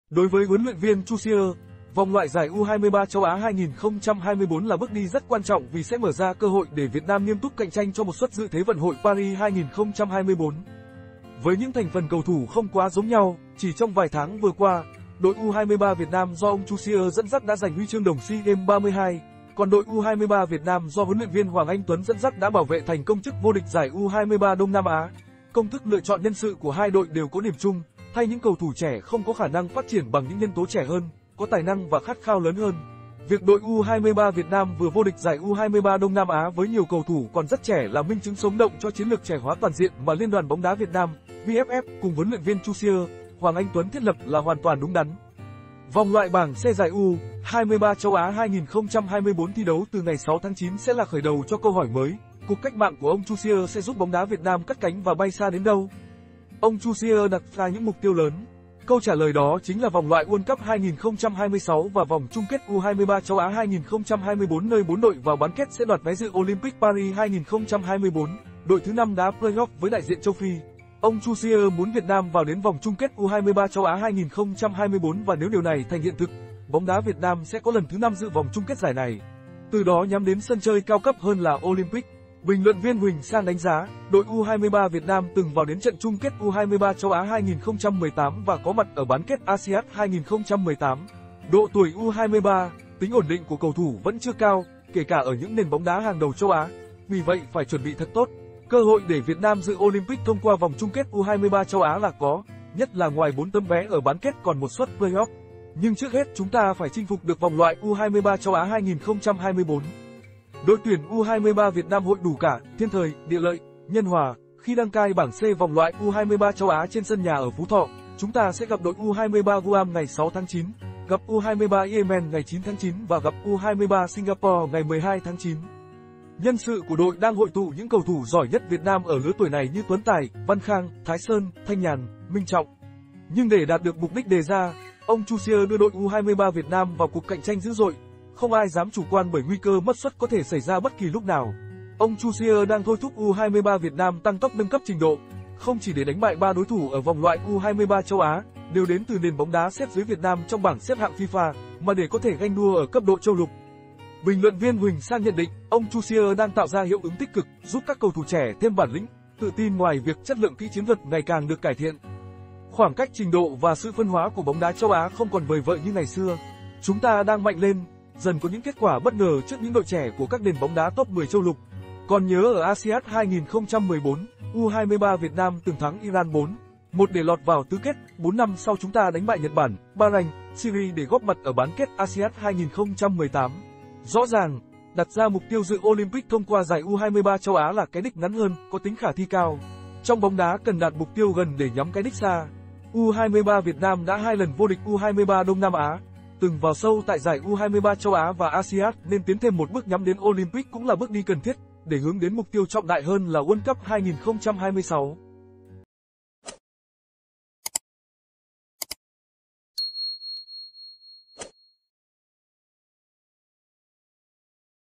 TẦM NHÌN OLYMPIC CHO U23 VIỆT NAM CỦA ÔNG TROUSSIER
Đối với huấn luyện viên Troussier, vòng loại giải U23 châu Á 2024 là bước đi rất quan trọng vì sẽ mở ra cơ hội để Việt Nam nghiêm túc cạnh tranh cho một suất dự Thế vận hội Paris 2024.